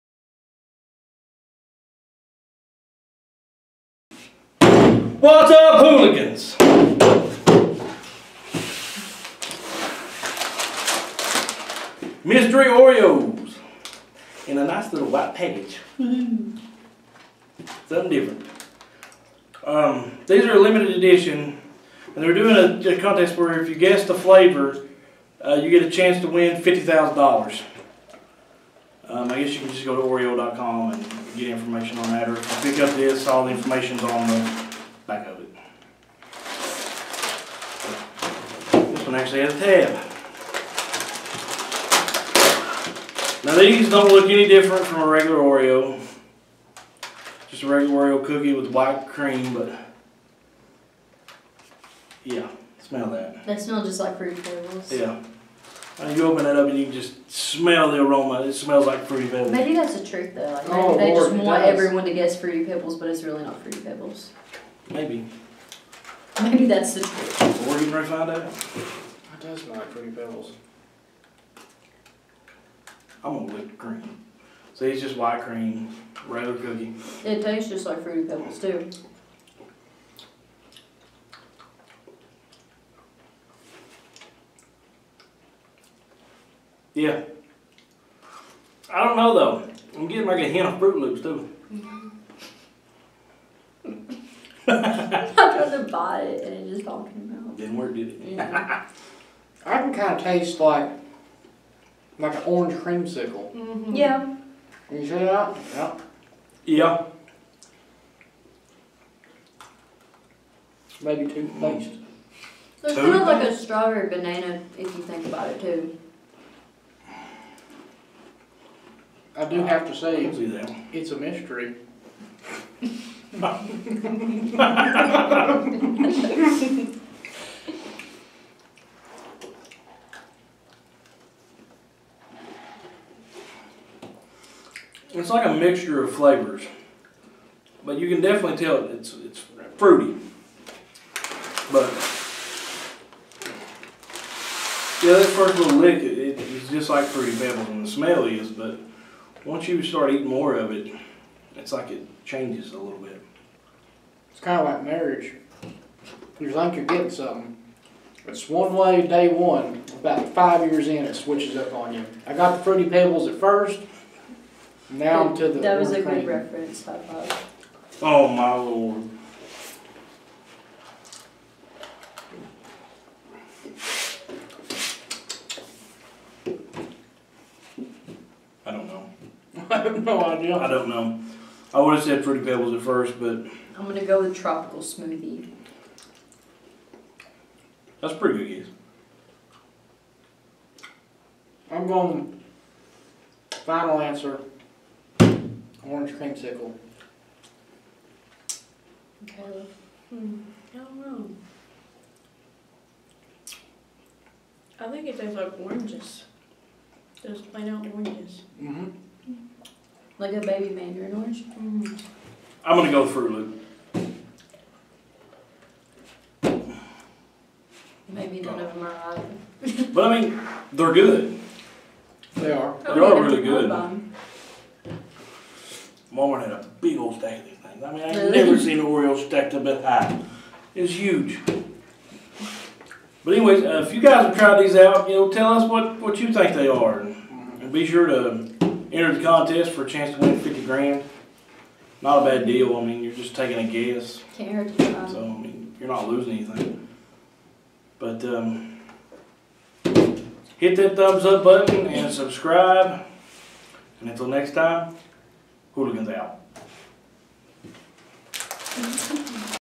Mystery Oreo Review
Can we guess what the  new mystery oreo flavor is?